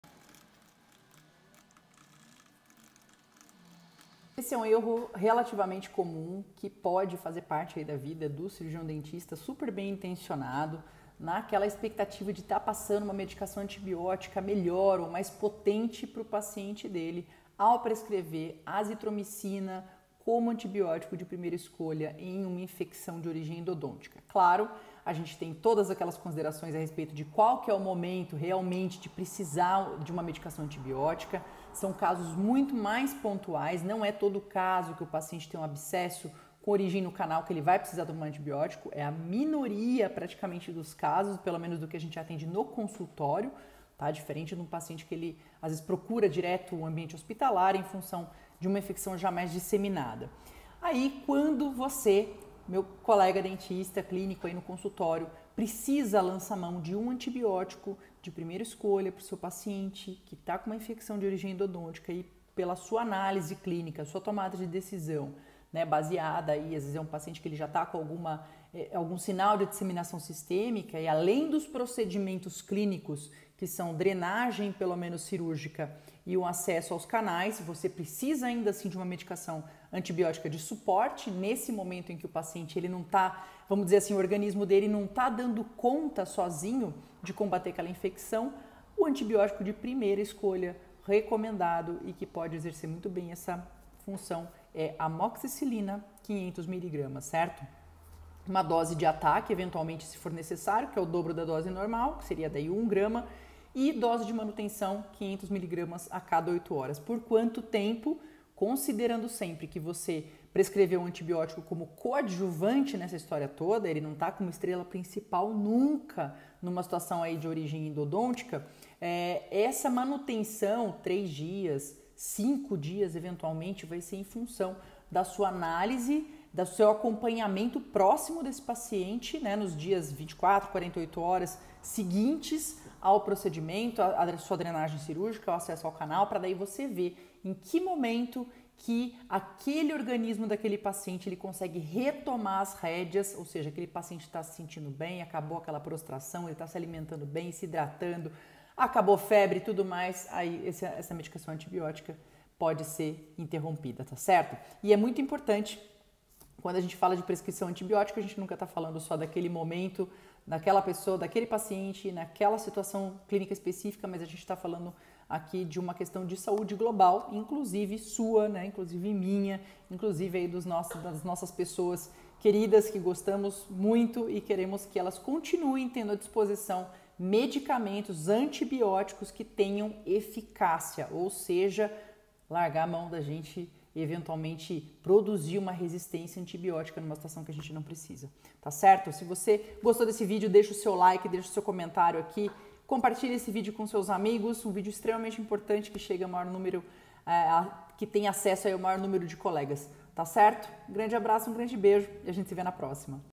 NÃO ERRE ESCOLHENDO ESSE ANTIBIÓTICO PRA ABSCESSO ENDODÔNTICO! - Endolovers
Saiba qual a medicação mais segura e confiável de primeira escolha em caso de antibiótico para abscesso de origem endodôntica!

Faça Já Sua Inscrição no Curso de Controle de Dor em Endodontia!